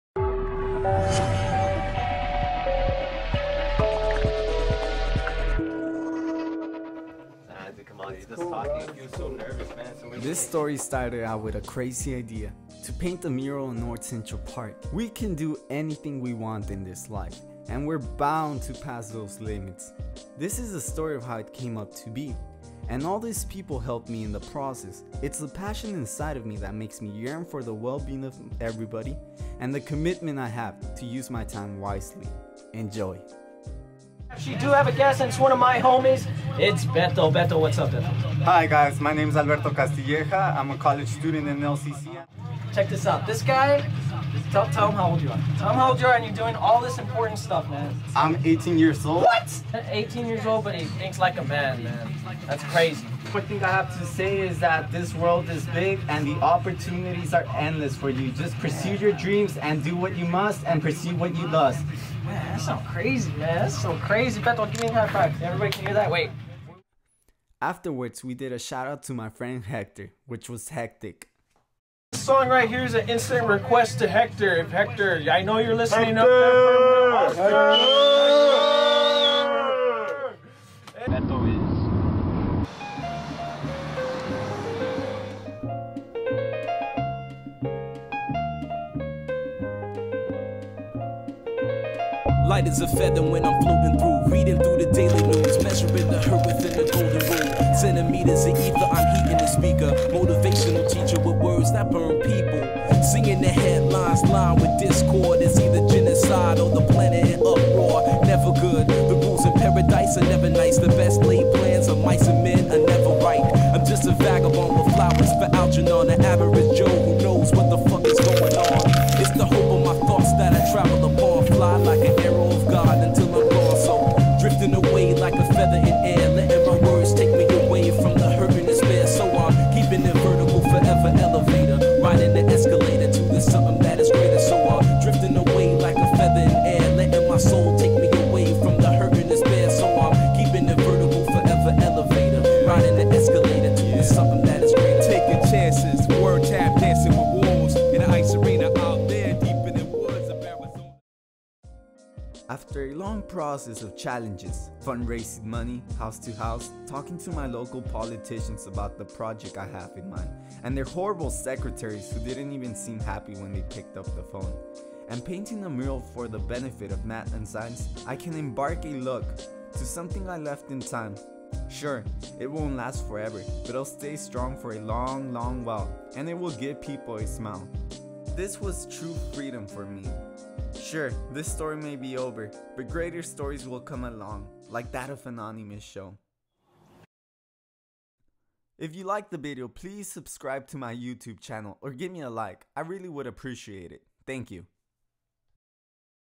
the world is mine (art project) Inspirational video
After having a dream one night to paint a mural in the benefit of math & science in north central park laredo I said why not...
I thanked everyone who funded my project ( Winners 016, 112, 019) This was just a small step in the transforming my landscape! I will proceed for more success stories.